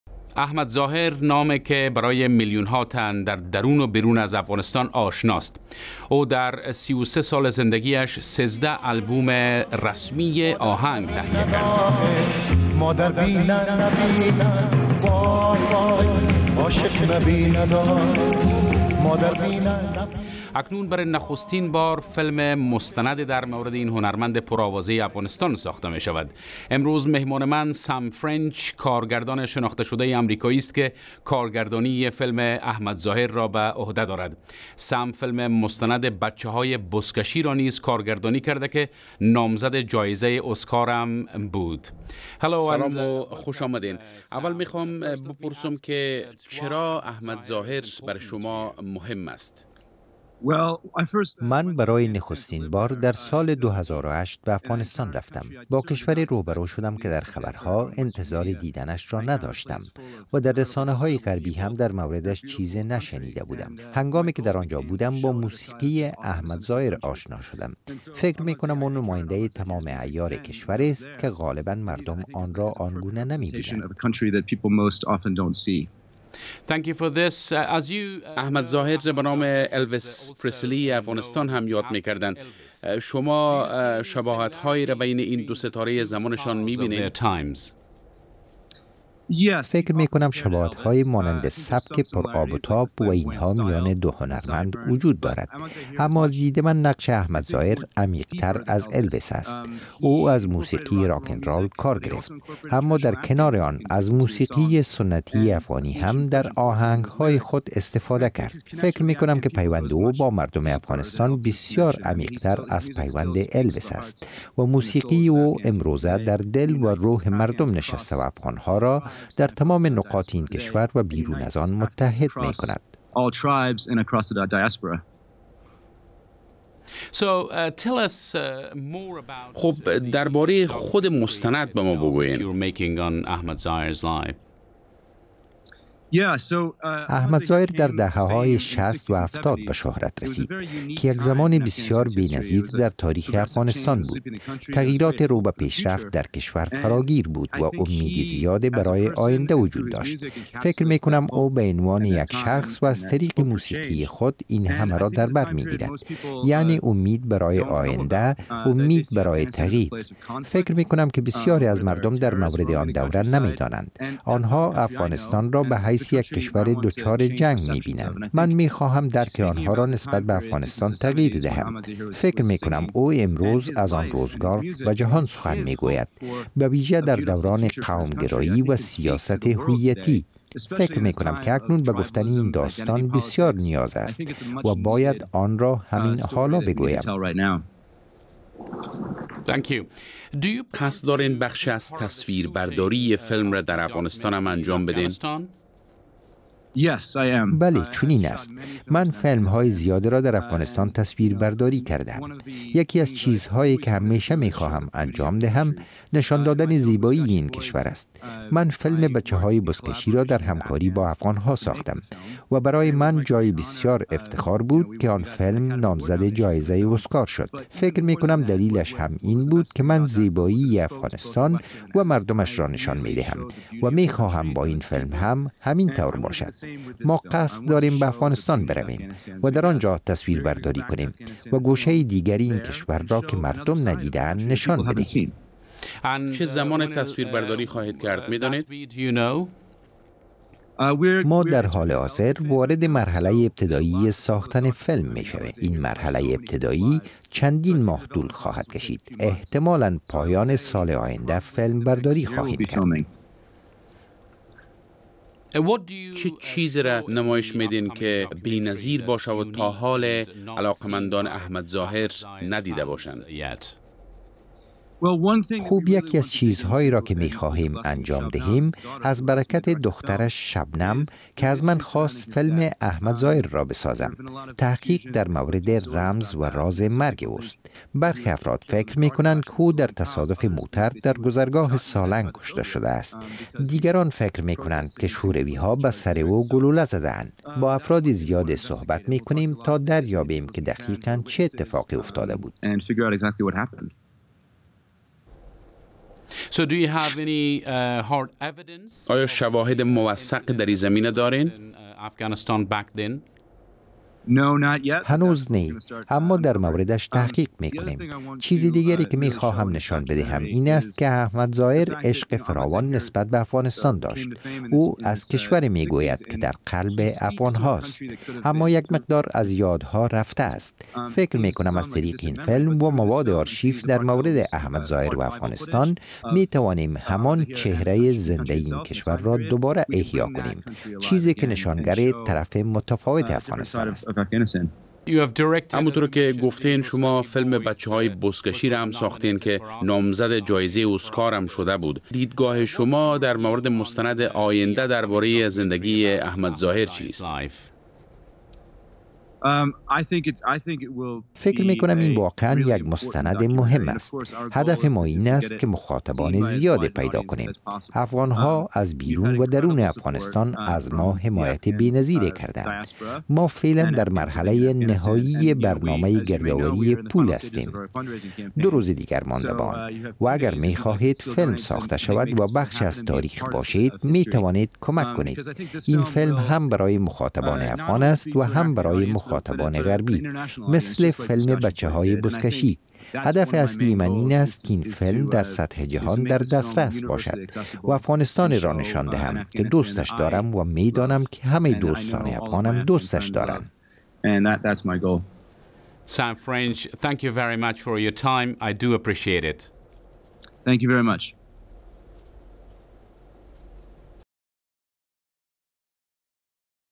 مصاحبه به فلمساز امریکایی که در بارۀ احمد ظاهر فلم میسازد
سم فرینج کارگردان امریکایی که با فلم "پسران بزکشی" به جایزهء اوسکار نامزد شده بود حالا قصد دارد در بارهء زندگی و مرگ احمد ظاهر معروفترین هنرمند افغان فلم بسازد. او در مصحبهء با مصطفی صدیقی در بارهء این فلم گفته و اینکه په زمانی فلمبرداری را آغاز خواهد کرد.